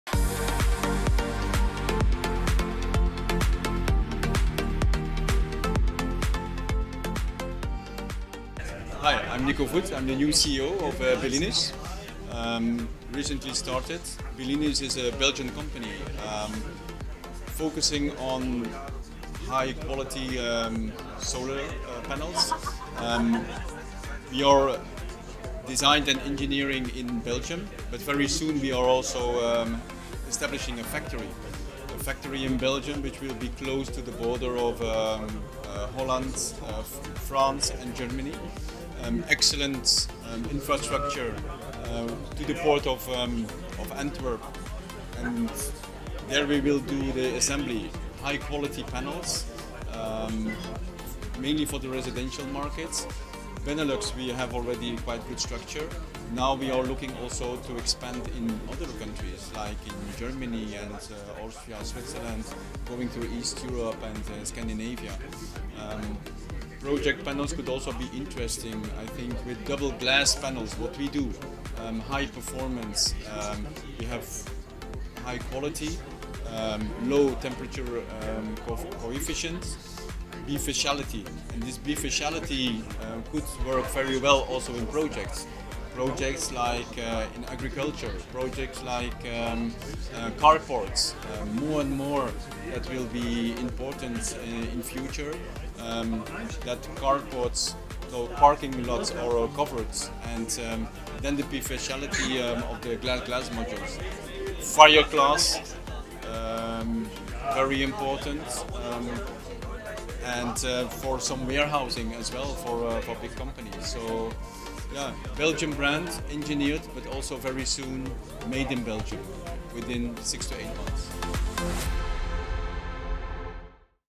Discover Belinus: Innovative Solar Panels and Energy Storage Batteries
The video showcases one of Belinus's latest innovations: the next-generation, ultra-thin solar panels. These high-quality panels efficiently generate sustainable energy, empowering individuals to transition to renewable energy sources in their own homes. Once again, Belinus reaffirms its commitment to advanced technologies and environmentally conscious solutions.